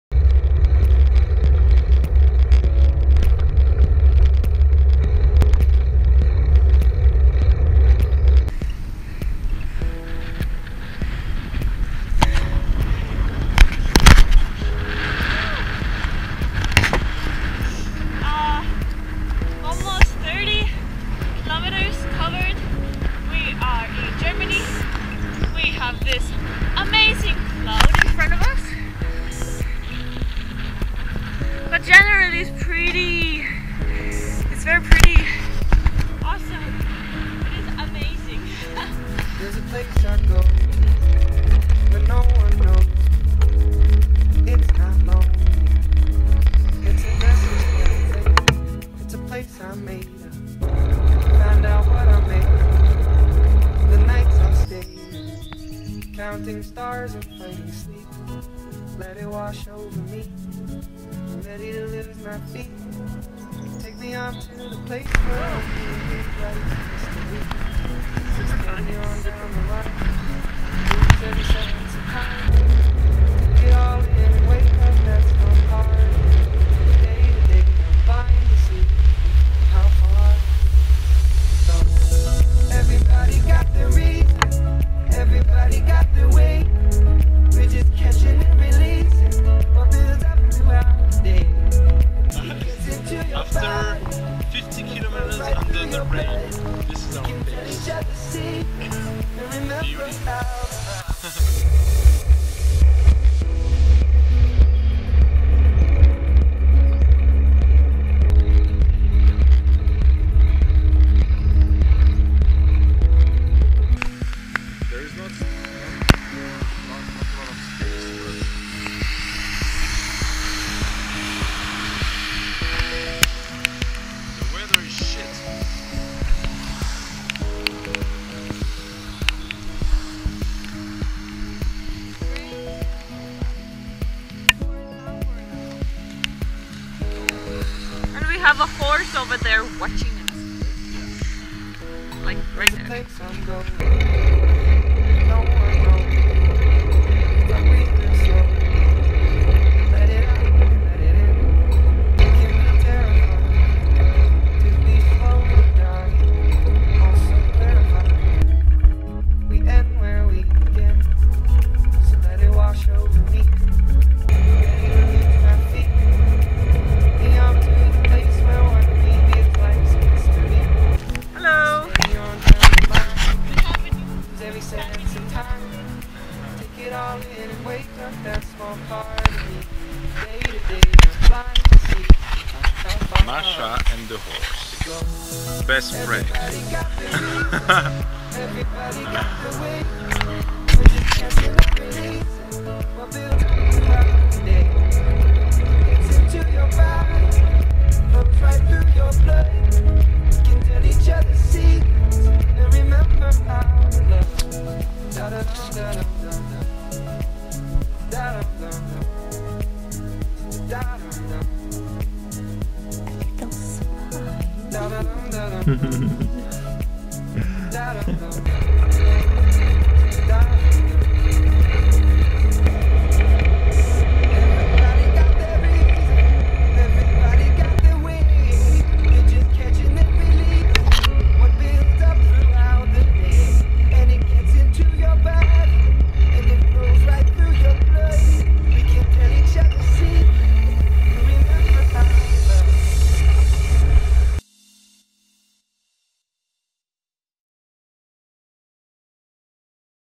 Biking through Germany
Part 2: Germany
Location: Puttgarden - Lübeck - Hamburg - Bremen - Osnabrück - Velne
Distance: 104,85 km - 96,98 km - 89,60 km - 125,78 km - 107,50 km
Days: 02/08 - 06/08/2016
Featuring: Maria Ovchinnikova, Rémi Nedelec